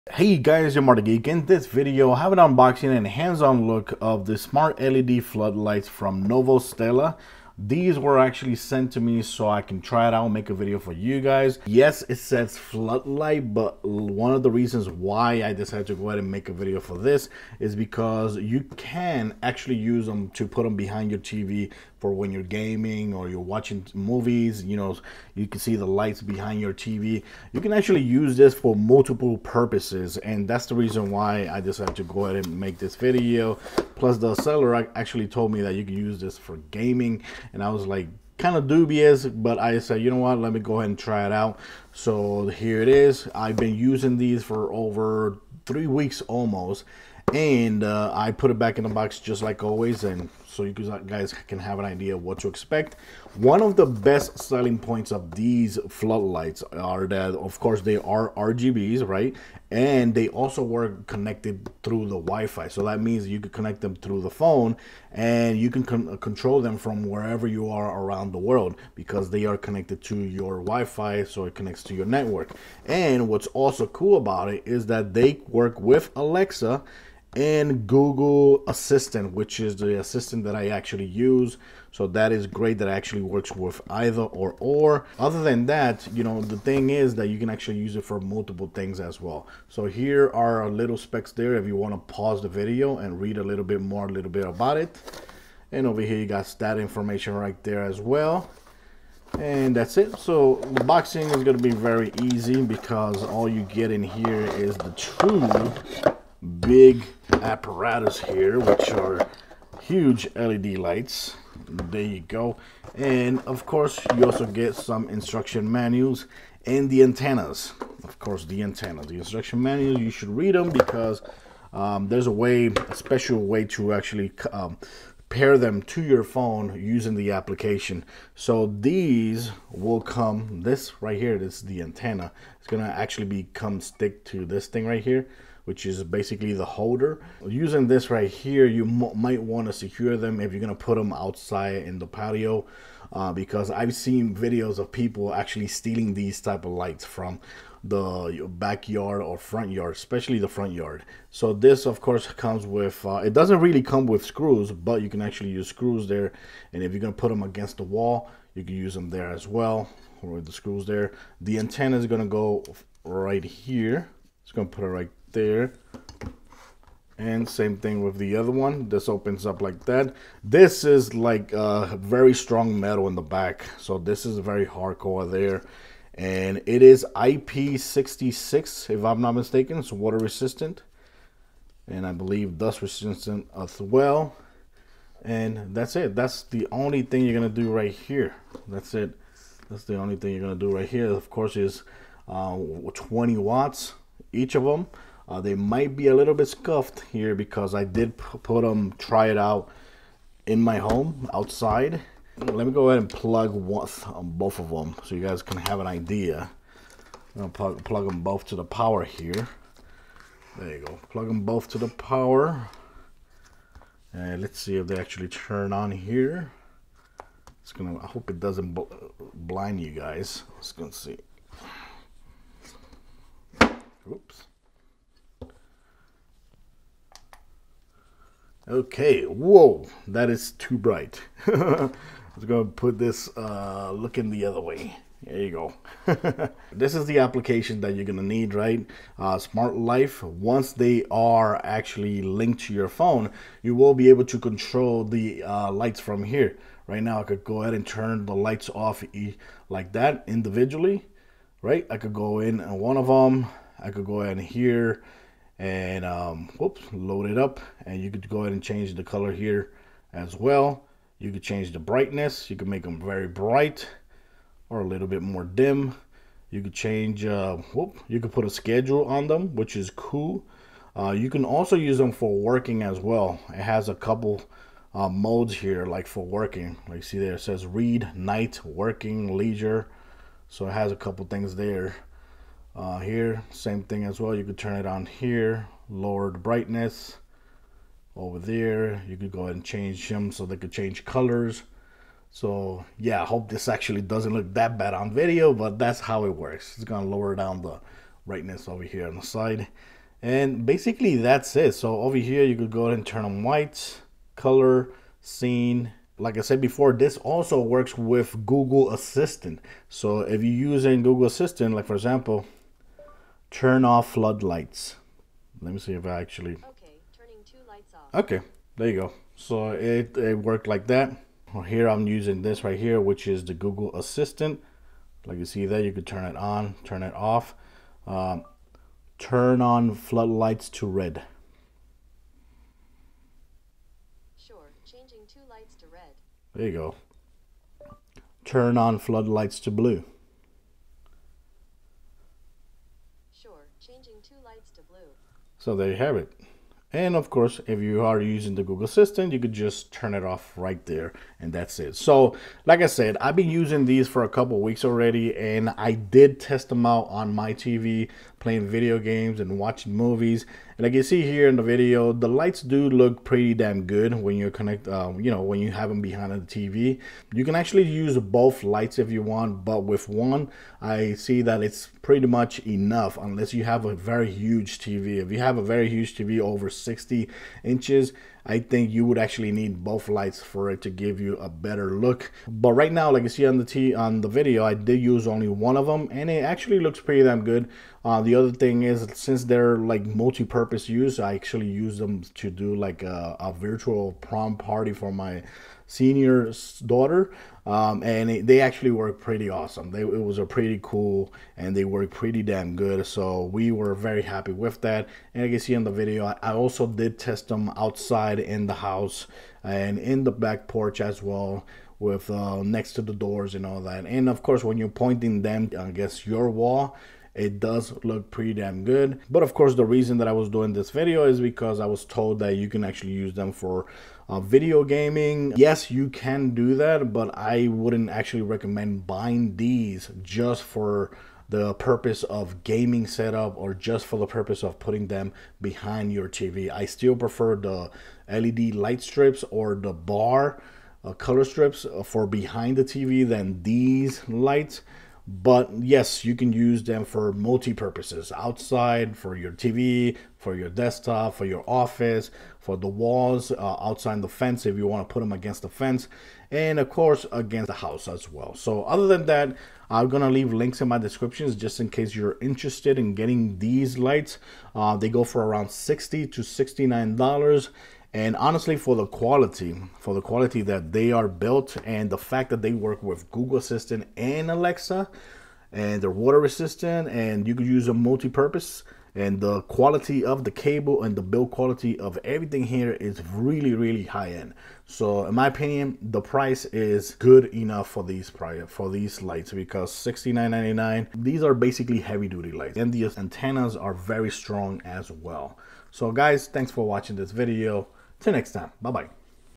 Novostella Multi-Purpose Smart LED Flood Lights
Here's a unboxing and hands on look at Novostella Flood RGB Smart Lights than can be Multi-Purpose and also work with Google Assistant and Alexa. These where sent to me so I can try and share with you, so here it is. Hope you like the video.